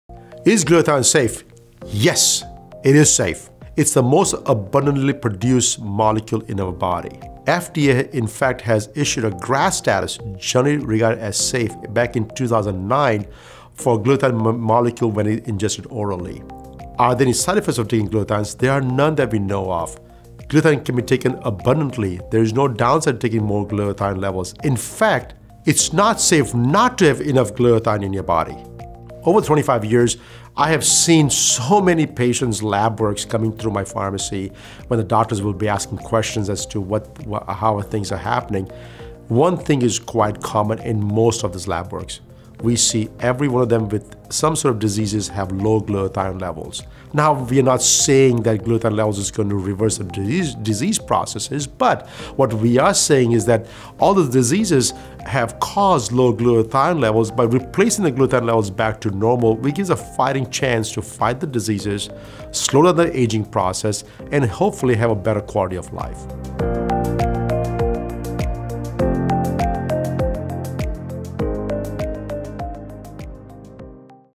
Is Glutathione Safe? | Auro Wellness
Is Glutathione Safe? Yes! Dr. Patel explains the Safety of Glutathione and explains that there are no downsides to taking an abundance of glutathione. In fact, it is actually not safe to not have enough Glutathione in your body. This is where Auro Wellness comes in.